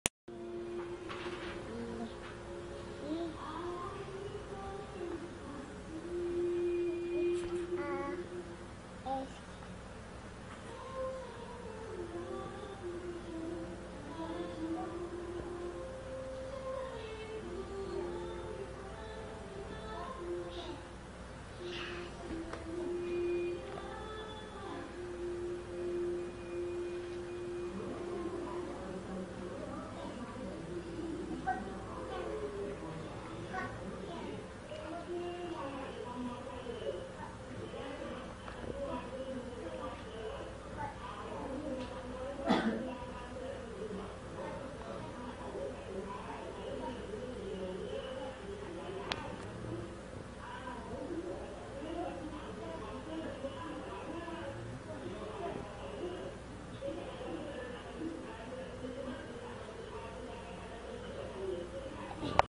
Hero Jilat Muka Aku | Hero Sayang Aku | Hero Dalam Kenangan
Sayang sangat Hero sama PH... Pemergiannya seperti baru berlaku semalam...